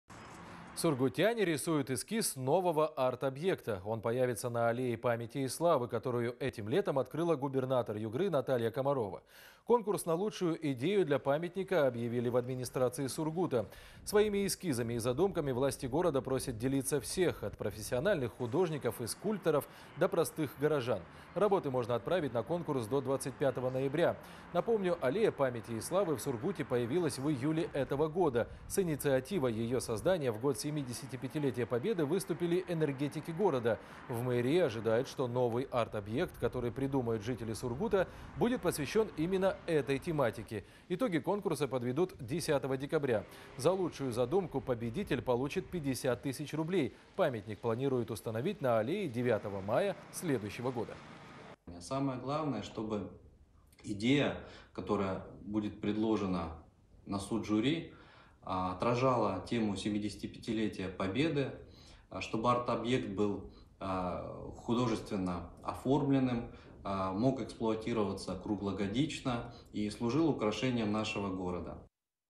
Сургутяне придумают новый памятник
Сургутяне рисуют эскиз нового арт-объекта. Он появится на Аллее памяти и славы, которую этим летом открыла губернатор Югры Наталья Комарова.